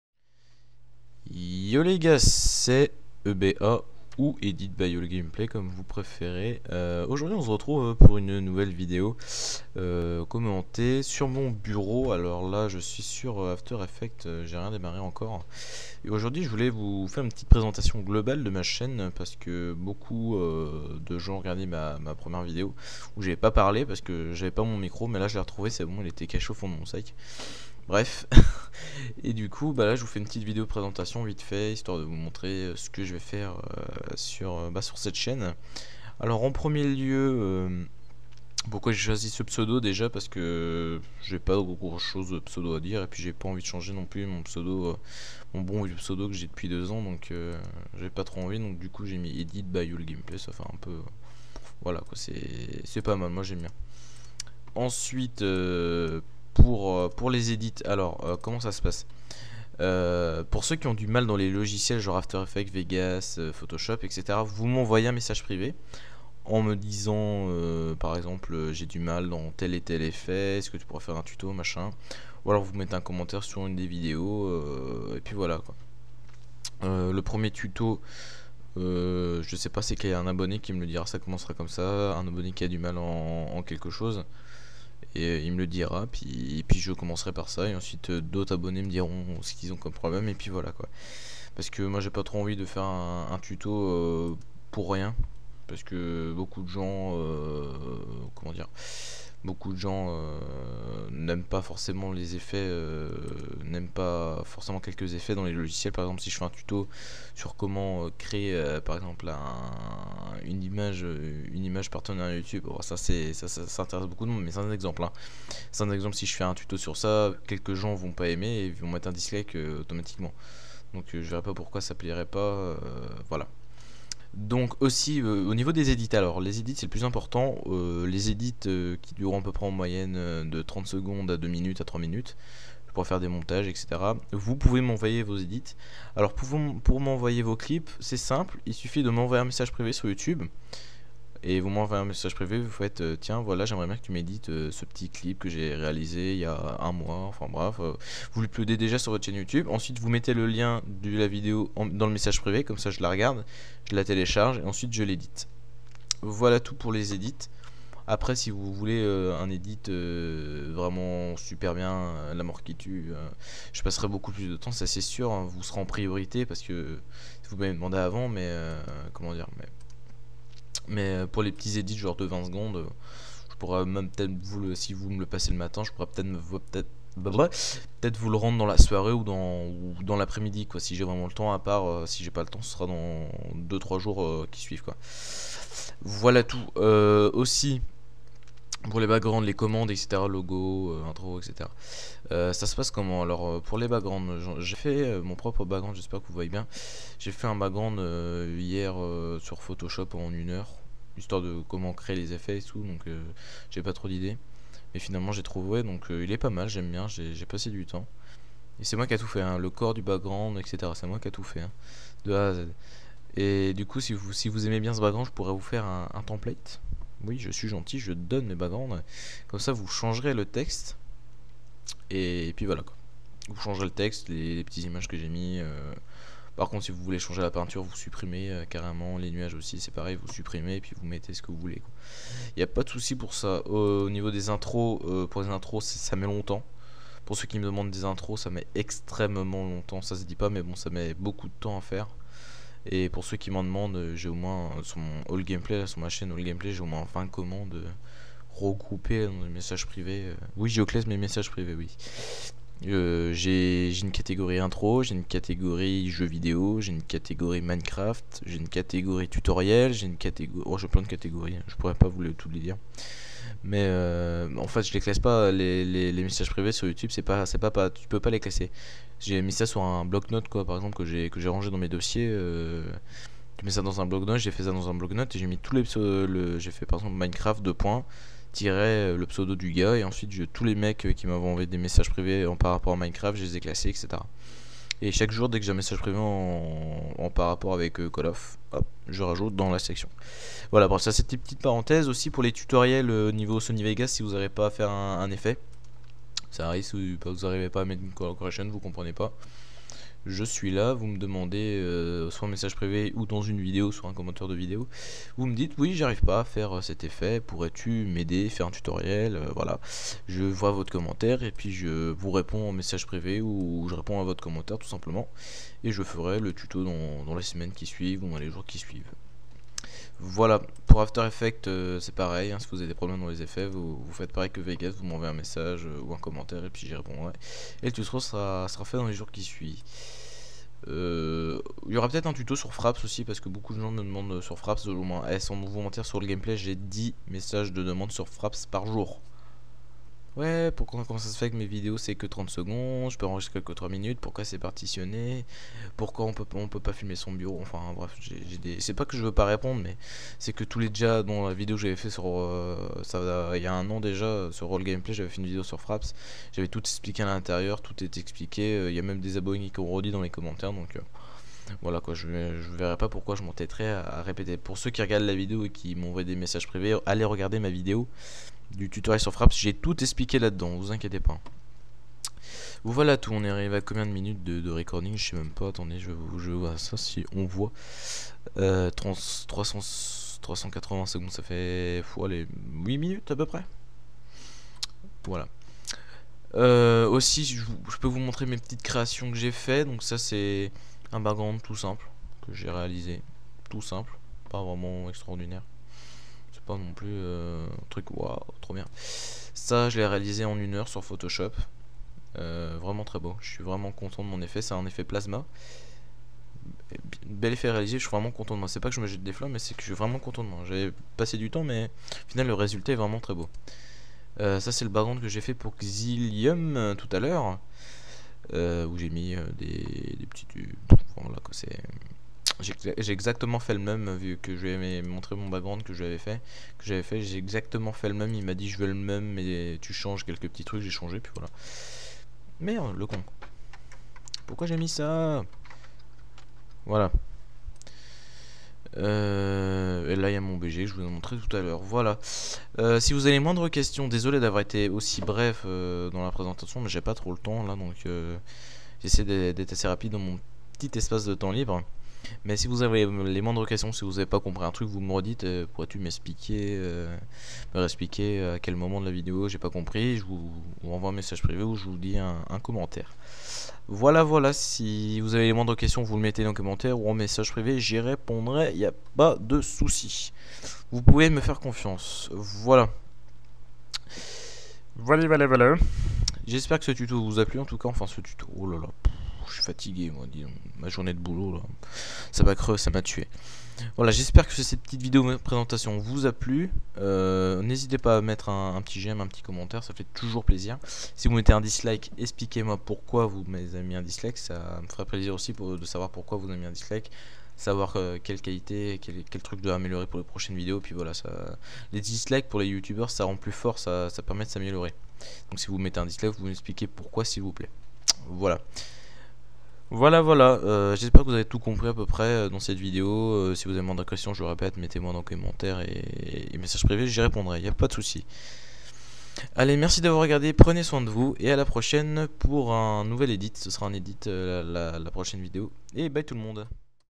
Presentation of EditByAllGameplays / Présentation d'EditByAllGameplays !
READ THE DESCRIPTION ! / LISEZ LA DESCRIPTION ! (Français plus bas)

Hello the lads!:)
Here is my last video which speaks about the presentation of my chain.
I explain quite from A to Z, listen to attentively the video, listen again to several times if you do not understand everything (I speak rather fast in the video it is parce - that I had not too much spare time; S).
To send me your clips so that I publish them it is simple: put your clip on YouTube, send a private message by saying that you want that I publish your clip and put it the link of your video, like that I download the video and I publish him later!;)
If you have the slightest questions concerning edicts, tutoriaux etc. rest I in private messages or in comments would answer me it without anybody concerns !;P Thanks in any case to have taken time to look at my video, if you have like, put a blue thumb, if you do not have like, put a red thumb. Put your impressions in comments also!;)
Ciao ciao for a new video.
Bye!

                            Cordially, EBA.

Voici ma dernière vidéo qui parle de la présentation de ma chaîne.
J'explique tout de A à Z, écoutez attentivement la vidéo, réécoutez la plusieurs fois si vous ne comprenez pas tout (je parle assez vite dans la vidéo c'est parce-que j'avais pas trop de temps libre ;S).
Pour m'envoyer vos clips pour que je les édite c'est simple :
Mettez votre clip sur YouTube, envoie un message privé en disant que vous voulez que j'édite votre clip et mettez-y le lien de votre vidéo, comme ça je télécharge la vidéo et je l'édite après ! ;)
Si vous avez les moindres questions concernant les édits, les tutoriaux etc.. posez les moi en messages privés ou en commentaires j'y répondrait sans aucun soucis ! ;P
Tchao tchao pour une nouvelle vidéo.
Bye !

                            Cordialement, EBA.